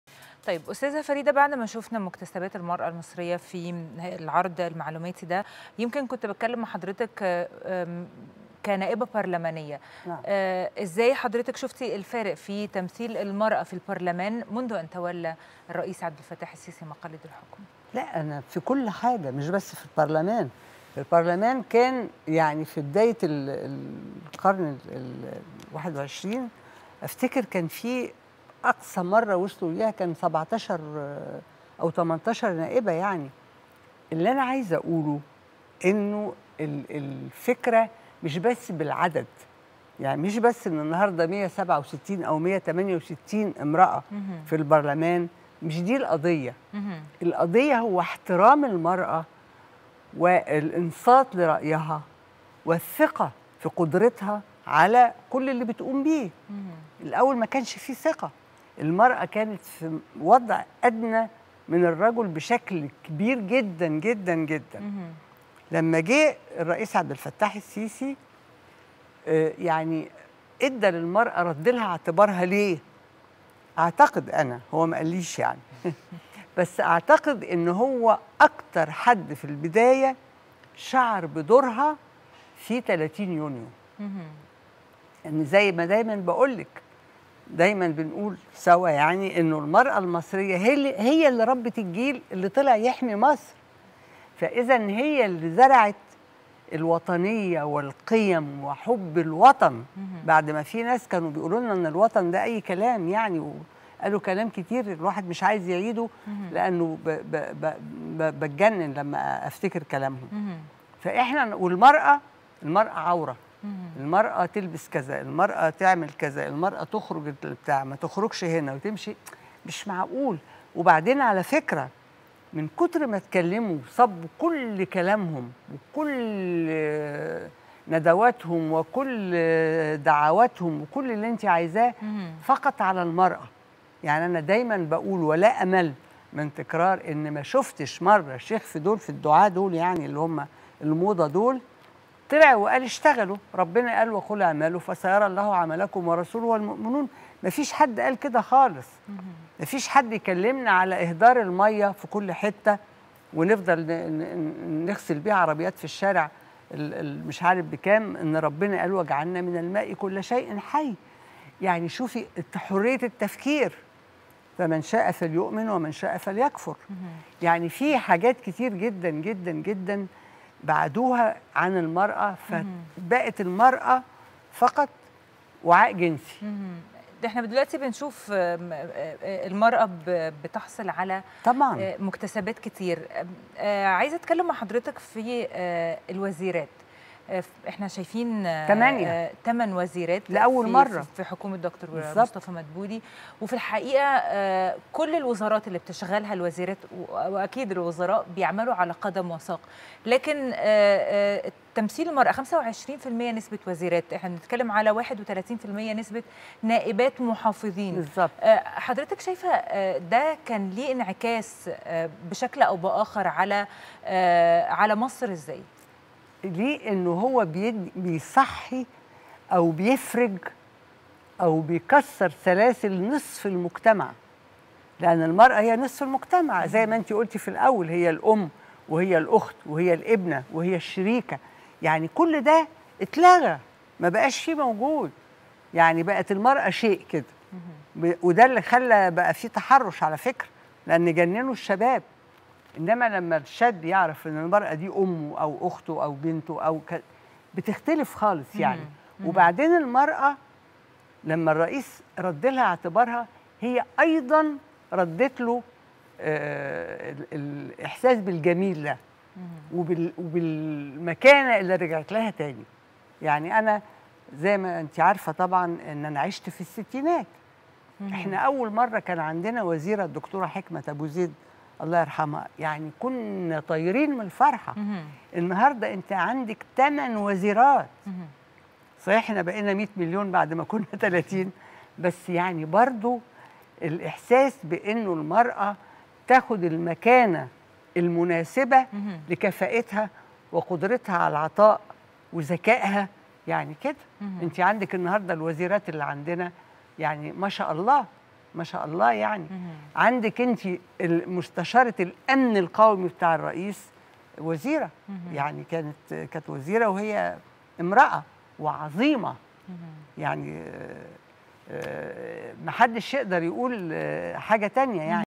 حصاد الأسبوع | مكتسبات غير مسبوقة.. فريدة الشوباشي: المرأة تعيش عصرا ذهبيا في عهد الرئيس السيسي
حصاد الأسبوع | مكتسبات غير مسبوقة.. فريدة الشوباشي: المرأة تعيش عصر ذهبي في عهد الرئيس السيسي